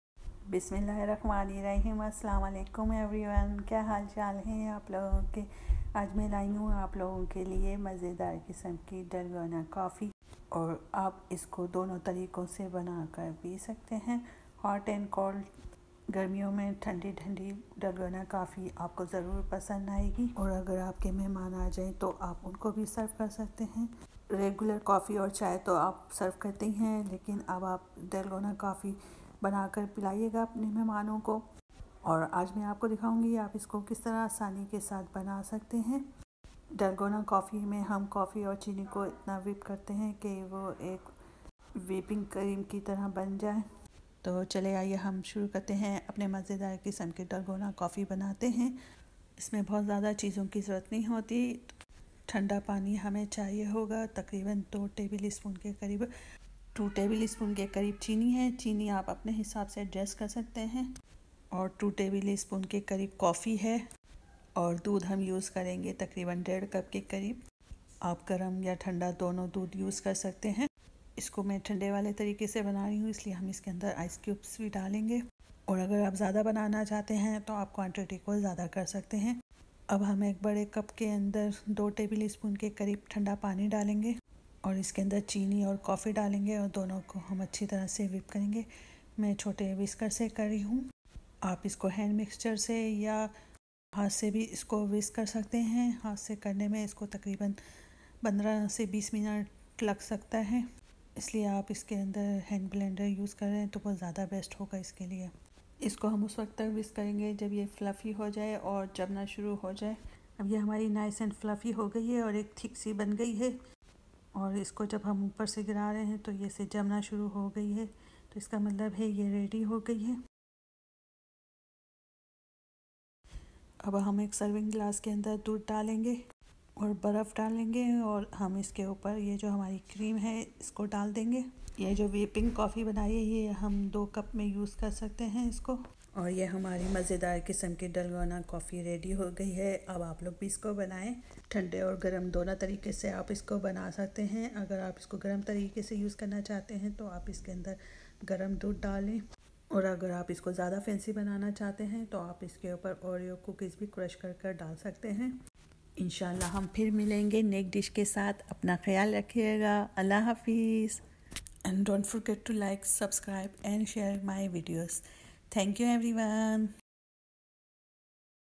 Let’s Make! Quick&Easy way😊 [ Dalgona Coffee ] 😊😋😍
Learn easy way how to make Dalgona coffee 😊
Ingredients
Instant Coffee 2 tbsp
Sugar 2 tbsp
Ice water 2 tbsp
Mix and whip till thick and fluffy

Milk cold /  hot as you like
If making cold coffee  add cold milk and Icecubes In cup put whip coffee on top  In swirl way enjoy 😊